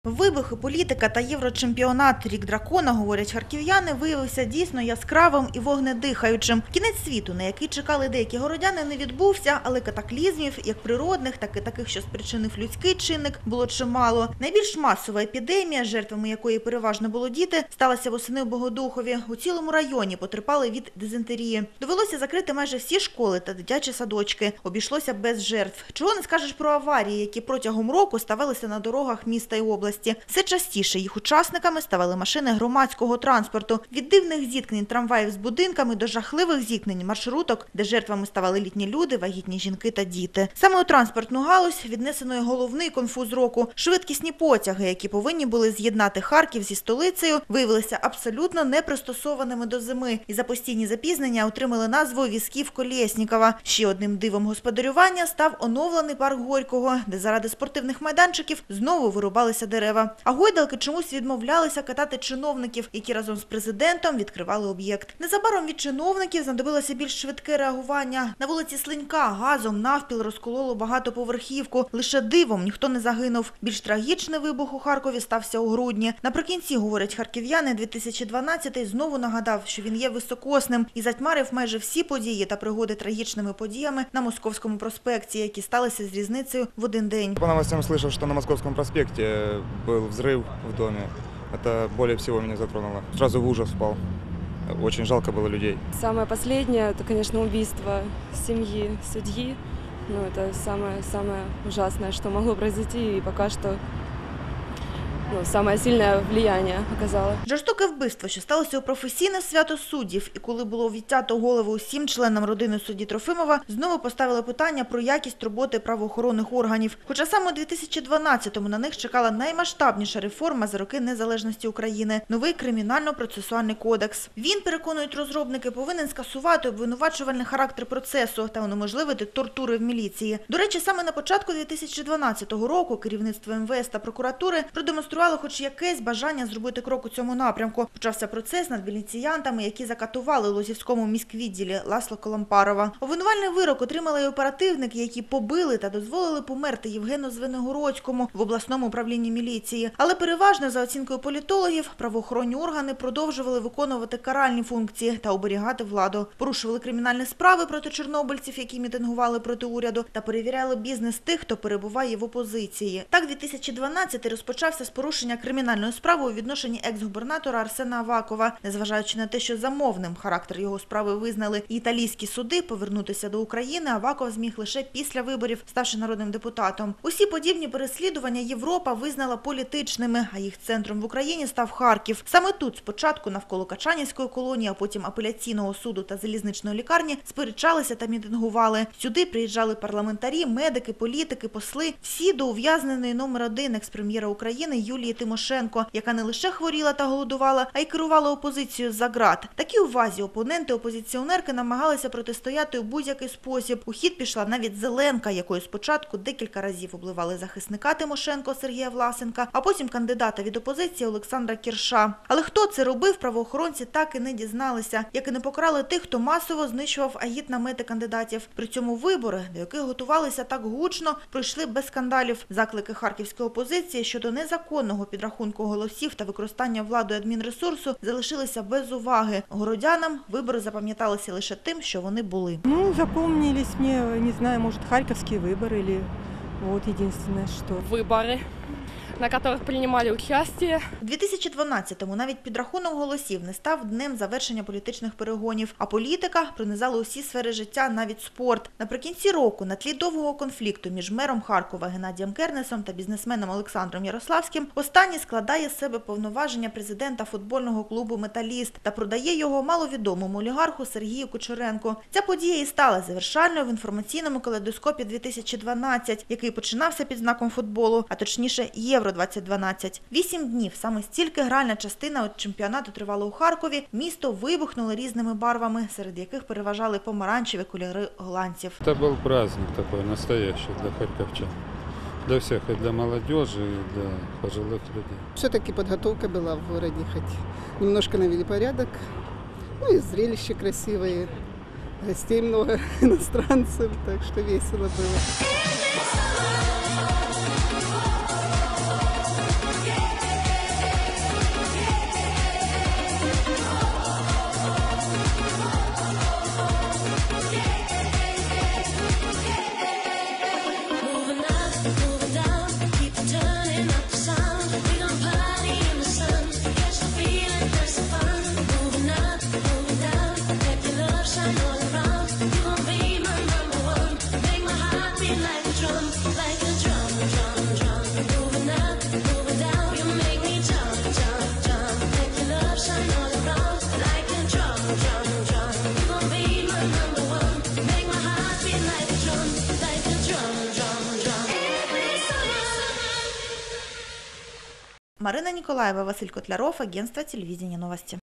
29.12.12 - Взрывы, политика и Еврочемпионат
Взрывы, политика и Еврочемпионат. Год Дракона, говорят харьковчане, оказался действительно ярким и огнедышащим.